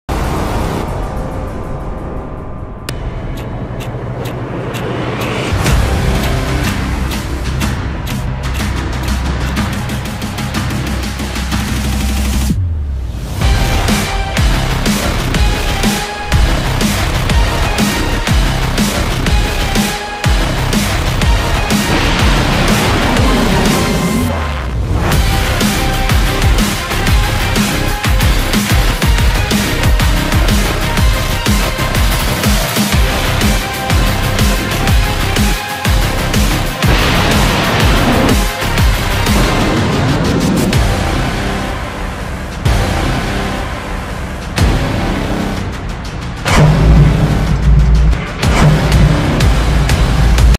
Atmaca Füzesinden Rekor Menzil! Hedefi 12'den Vurdu.
Roketsan tarafından yerli ve milli imkanlarla geliştirilen Kara Atmaca Karadan Karaya Seyir Füzesi, en uzun menzilli ve en uzun süreli uçuşunda hedefi tam isabet vurdu.
ATMACA UM atışlı test faaliyetinde hedefini tam isabet vurarak, en uzun menzilli ve en uzun süreli uçuşunu gerçekleştirdi. Ülkemize ve aziz milletimize hayırlı olsun" ifadelerine yer verildi.